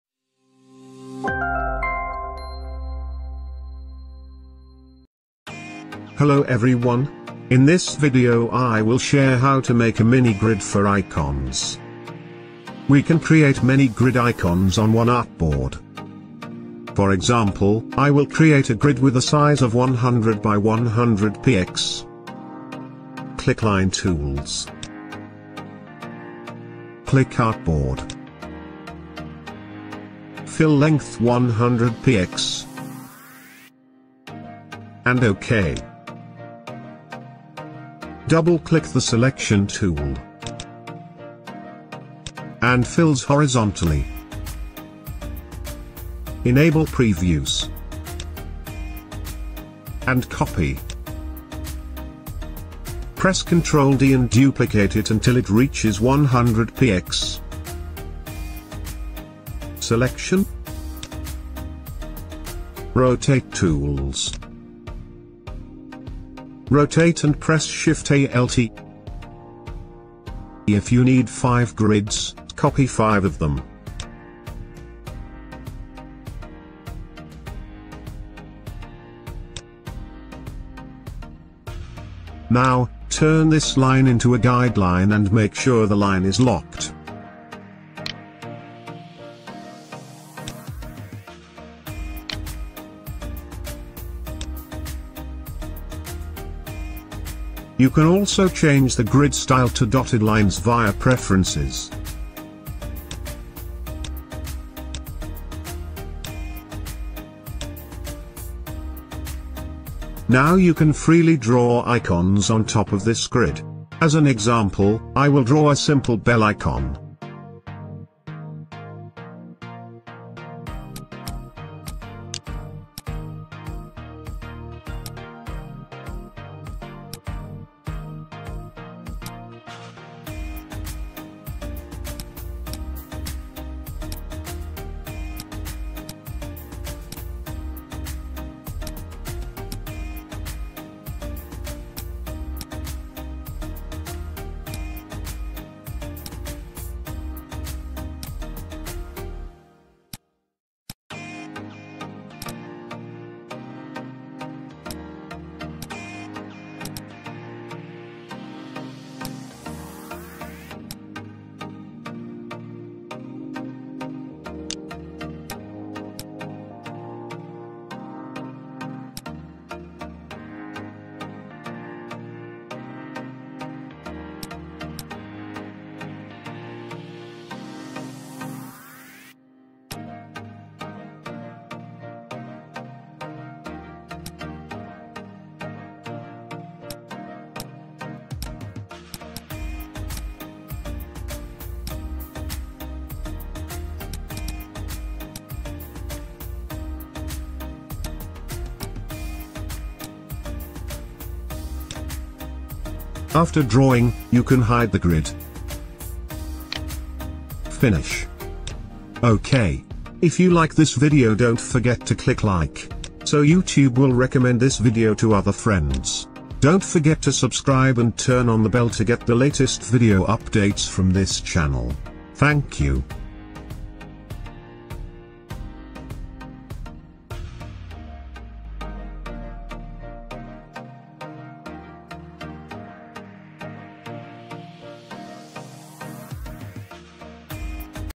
How to Make Grid For Icon - Adobe Illustrator Tutorial
working with grids will make the icons more precise and neater. there are actually many ways to work with grids, one of which is like the one in this video.